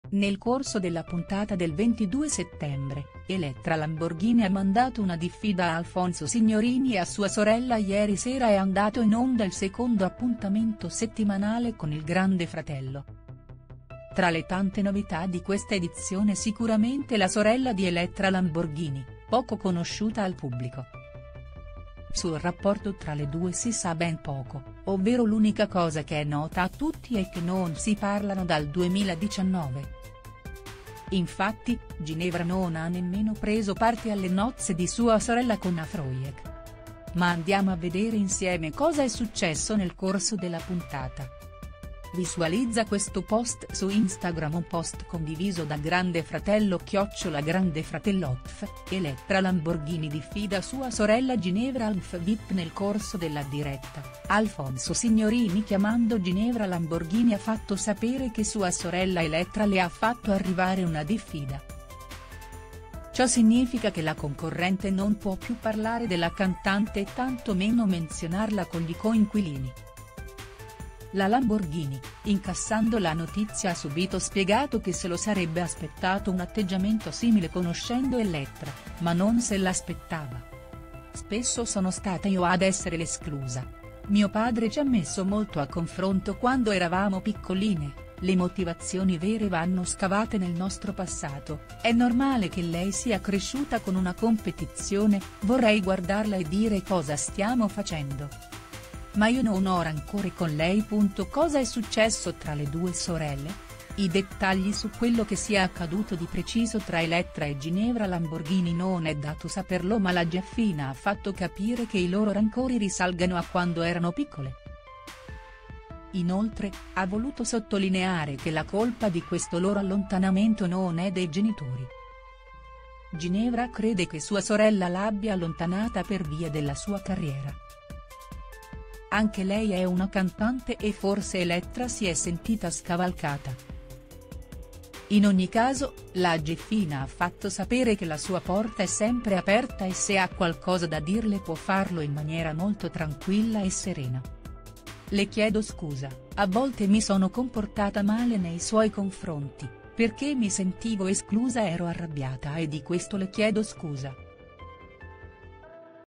Grande Fratello Vip: Elettra Lamborghini diffida sua sorella Ginevra, lei: "Me l'aspettavo"
Nel corso della puntata del 22 settembre, Elettra Lamborghini ha mandato una diffida a Alfonso Signorini e a sua sorella Ieri sera è andato in onda il secondo appuntamento settimanale con il Grande Fratello. Tra le tante novità di questa edizione sicuramente la sorella di Elettra Lamborghini , poco conosciuta al pubblico. Sul rapporto tra le due si sa ben poco, ovvero l’unica cosa che è nota a tutti è che non si parlano dal 2019. Infatti, Ginevra non ha nemmeno preso parte alle nozze di sua sore